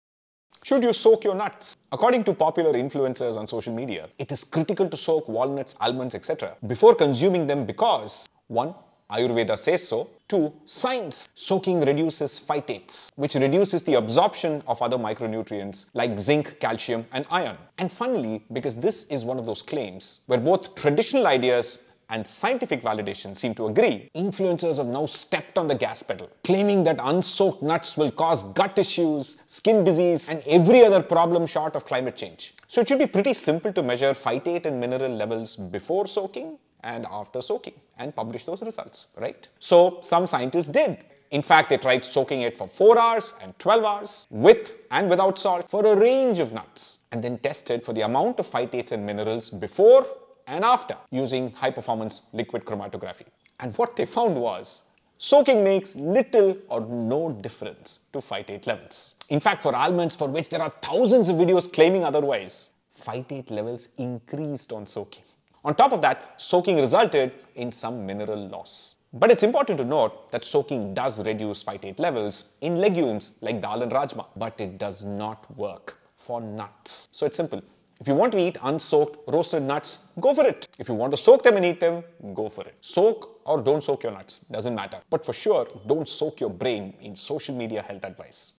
Should you soak your nuts ?
According to influenzas, not soaking your nuts is a recipe for global catastrophe. Here is what the science says though